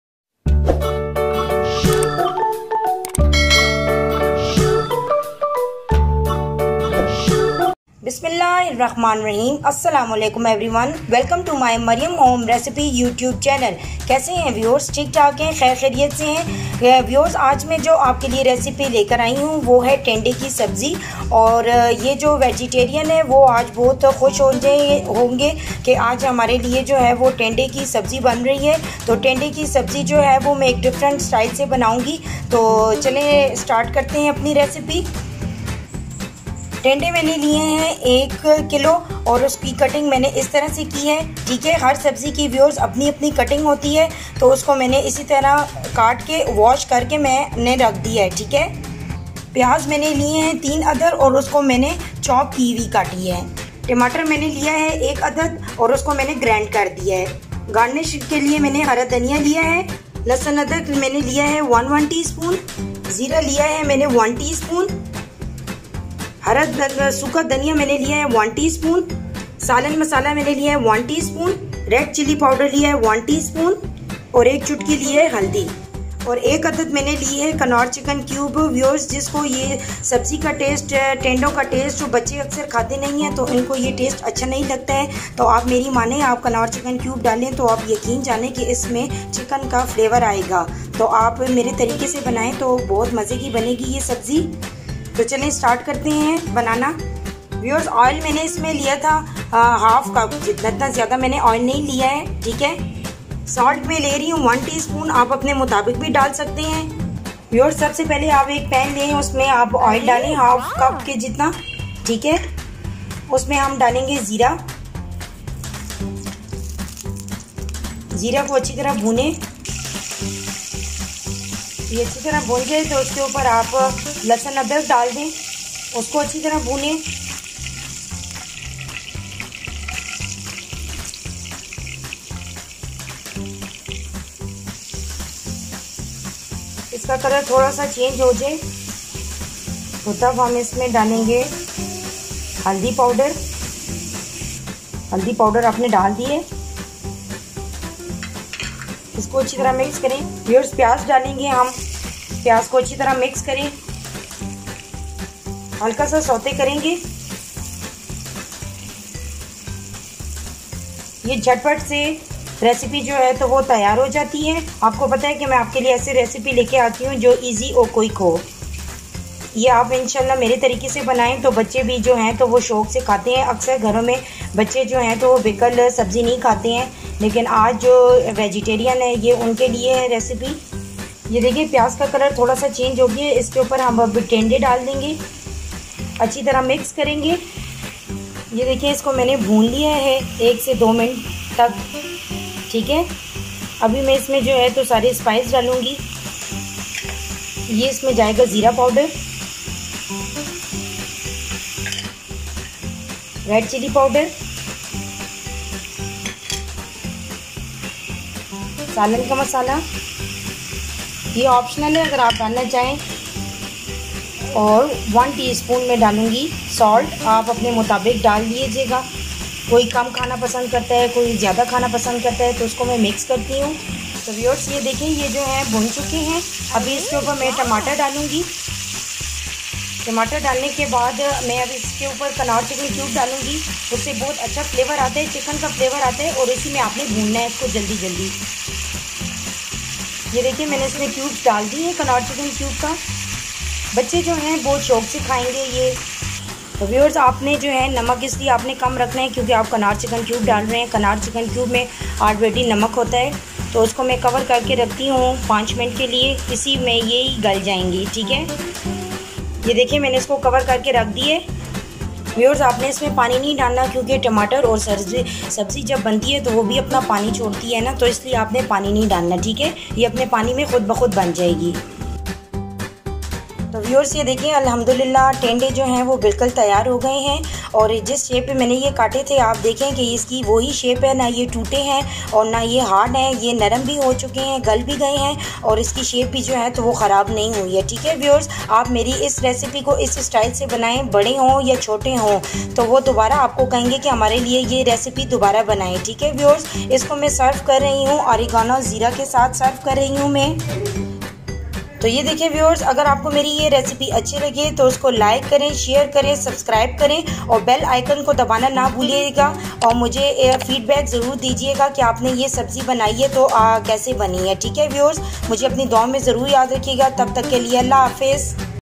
Tandy masala recipe/Tandy ki sabzi/Tandy ka salan.
It's a delicious and nutrition vegetable recipe. It's a tasty & quick affort able recipe.